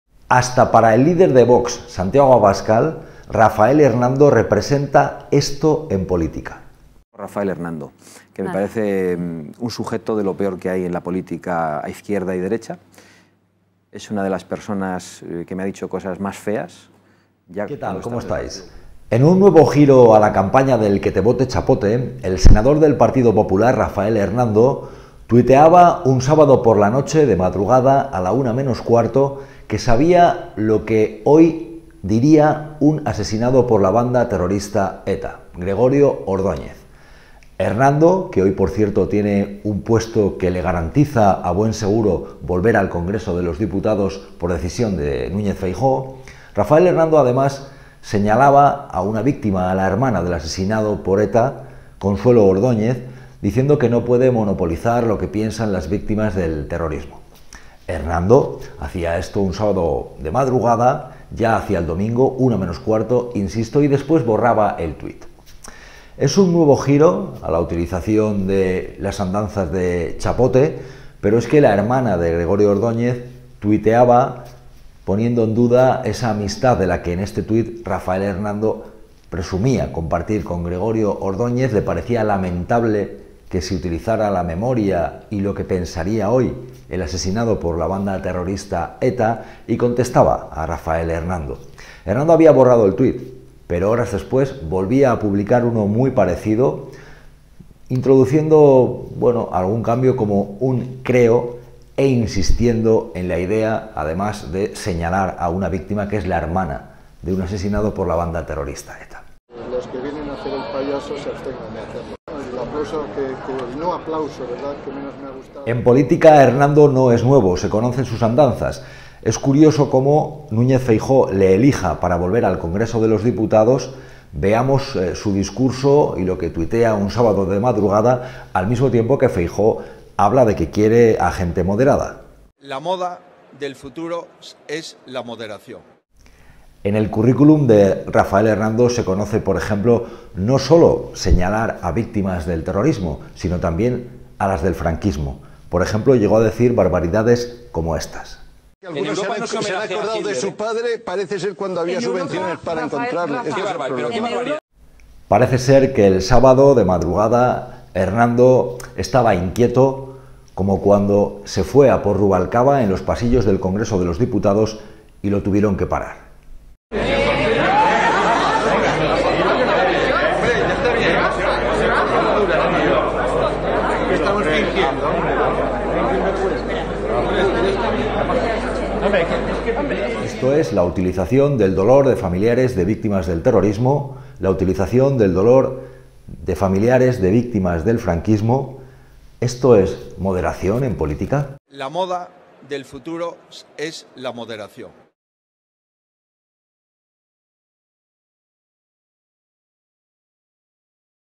Rafael Hernando contra familiares de víctimas: “Que te vote Txapote”. Consuelo Ordoñez señalada
Rafael Hernando (PP) contra familiares de víctimas. Escribe un sábado de madrugada lo que según él diría un asesinado por ETA a Sánchez: “Que te vote Txapote”. La hermana de la víctima, Consuelo Ordoñez, señalada. Hasta Abascal ya dijo que Hernando es “de lo peor”…